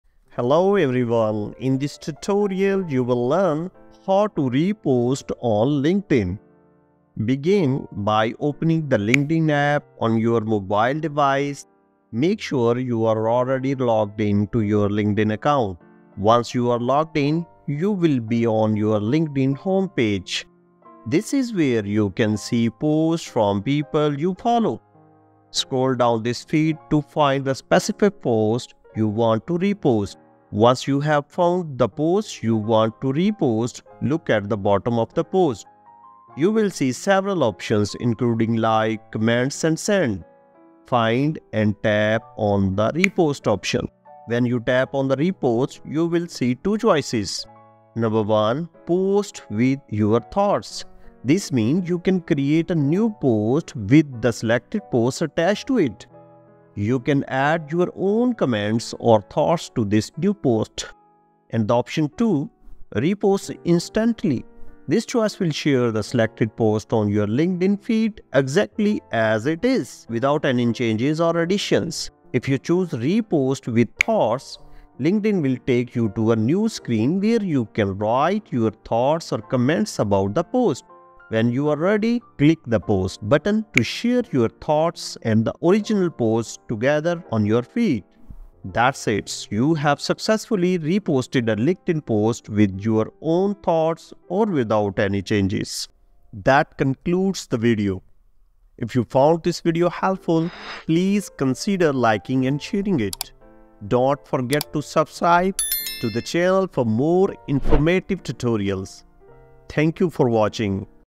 How to Repost on LinkedIn App (Step-by-Step Process)
How to Repost on LinkedIn App (Step-by-Step Process). In this tutorial, you will learn how to repost on LinkedIn App.